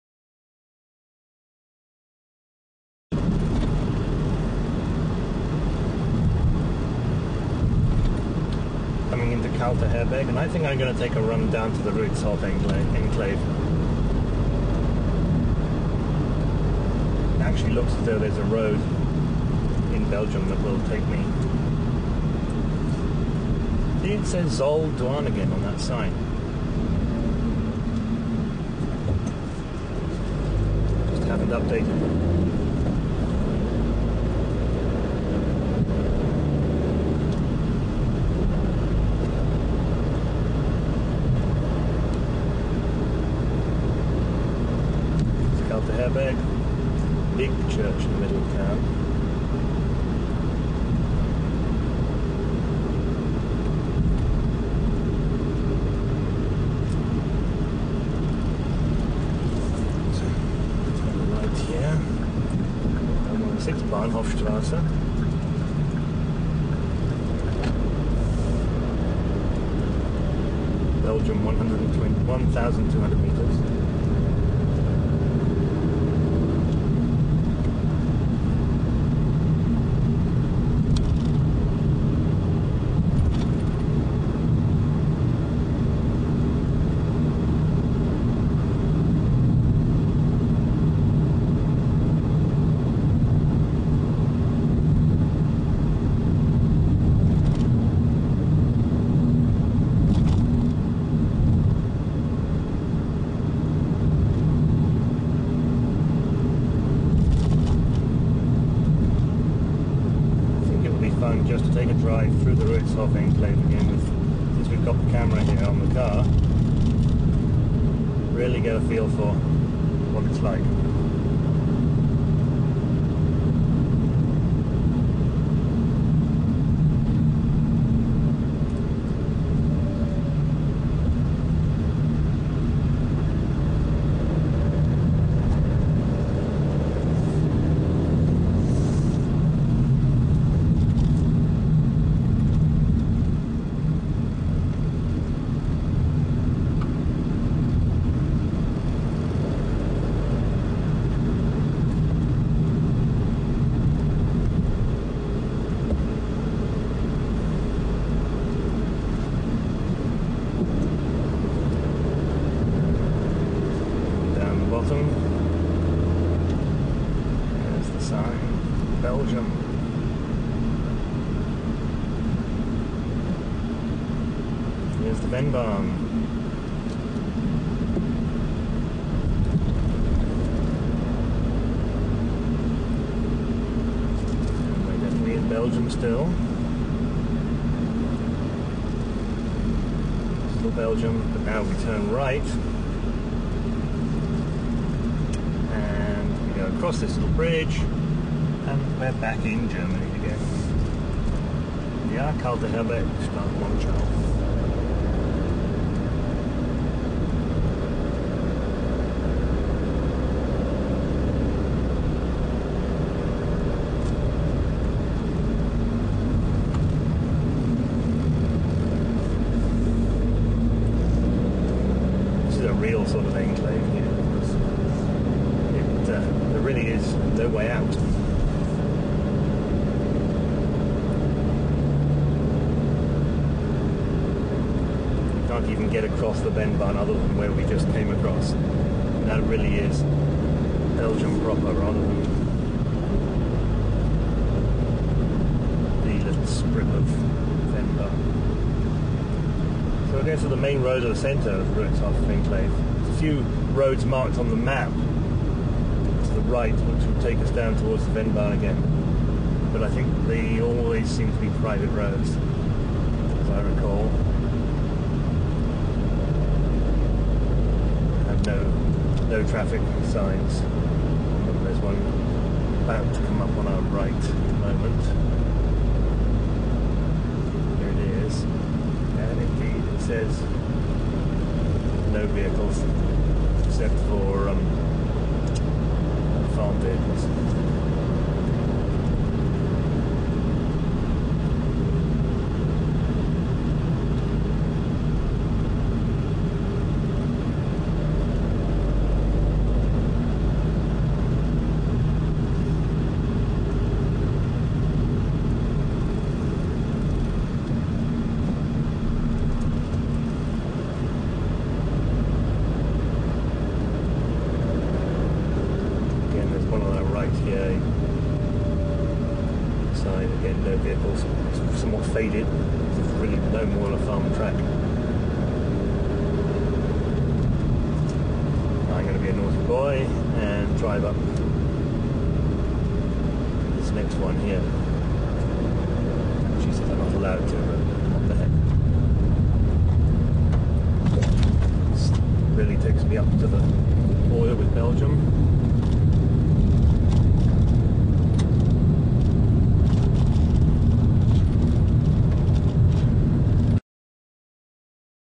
Kalterherberg-Ruitzhof enclave
Driving from Kalterherberg through the Ruitzhof enclave(formed by the Vennbahn).